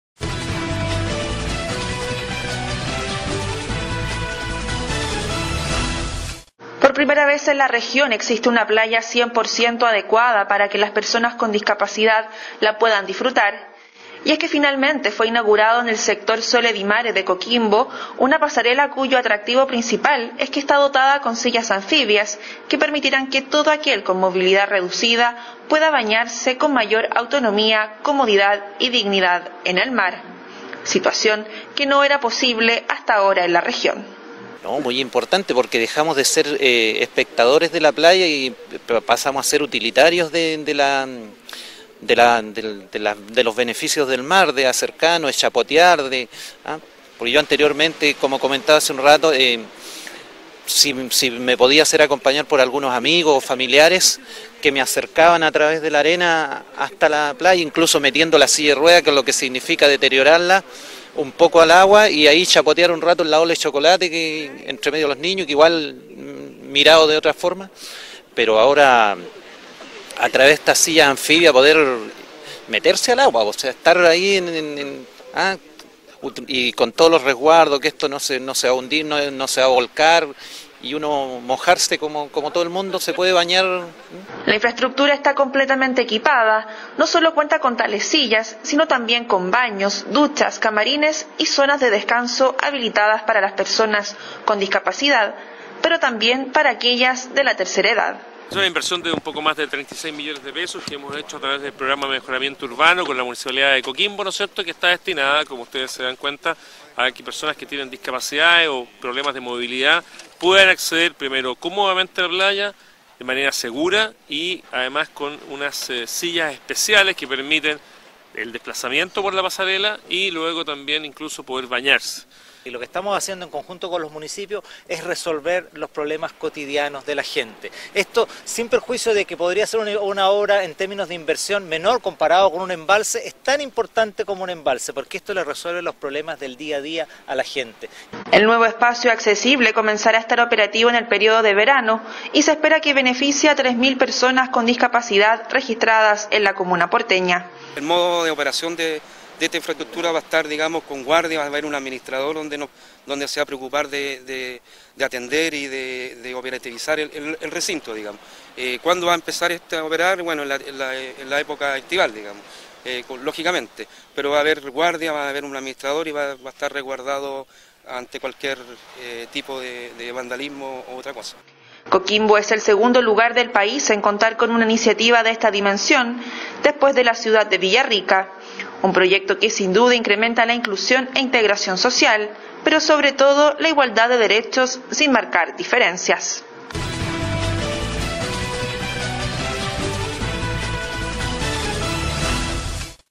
Mobi-mat® - Inauguran área de acceso a la playa para personas con discapacidad
Mobi-mat® presents you an amazing video about the inauguration of Mobi-chair™ floating wheelchairs.

Por primera vez en la region existe una playa 100% adecuada para que las personas con discapacidad la puedan disfrutar y es que finalmente fue inaugurado en el sector soledad de coquimbo. Una pasarela cuyo atractivo principal es que esta dotada con ensillas anfibias que permitiran a todo aquel como movilidad reducida, pueda banarse con mayor autonomia, comodidad y dignidad en el mar; situacion que no era posible hasta hora en la region.

- Lo muy importante es que dejamos de ser espectadores de la playa y pasamos a ser utilitarios de los beneficios del mar, de acercanos, chapotear. Anteriormente como comentaba hace un rato, si me podia hacer acompanar por algunos amigos, familiares que me acercan atraves de la arena hasta la playa incluso metiendo la silla de rueda que lo significa deteriorar la, un poco al agua y ahi chapotear un rato el lado de chocolate que entremedio los ninos igual mirado de otra forma pero ahora atrafves de esta silla anfibia poder meterse al agua, estar ahi y con todos reguardos que esto no sea un digno, no sea volcar y uno mojarse como todo el mundo puede se banar.

La infrustructura esta completamente equipada, no solo cuenta con tales sillas sino tambien con banos, duchas, camarins y zonas de descanso habilitadas para las personas con discapacidad; pero tambien para aquellas de la tercer edad.

- Una inversion de un poco mas de 36 millones de pesos que hemos hecho a traves del programa de mejoramiento urbano con la municipalidad de coquimbo. Nos excepto que esta destinada como ustedes se dan cuenta aqui personas que tienen discapacidades o problemas de movilidad, pueden acceder primero comodamente en la playa de manera segura y ademas con unas sillas especiales que permiten el desplazamiento por la pasarela y luego tambien incluso poder banarse.

- Lo que estamos haciendo en conjunto con los municipios es resolver los problemas cotidianos de la gente, esto sin perjuicio de que podria ser una obra en terminos de inversion menos comparado con un embalse, es tan importante porque esto le resuelve los problemas del dia a dia a la gente.

El nuevo espacio accesible comenza a estar operativo en el periodio de verano y se espera que beneficio a 3000 personas con discapalidadregistradas en la comuna portena.

- El modo de operacion de infraestructura va a estar digamos con guardias, va a haber un administrador donde sea preocupar de atender y de gobernar. Cuando va a empezar este operar en la epoca estival con logicamente pero va a estar reguardado ante cualquier tipo de vandalismo.

Coquimbo es el segundo lugar en pais en contar con una iniciativa de esta dimension despues de la ciudad de Villarrica; un proyecto que sin duda incrementa la inclusion e integracion social. Pero, sobre todo la igualdad de derechos sin marcar diferencias.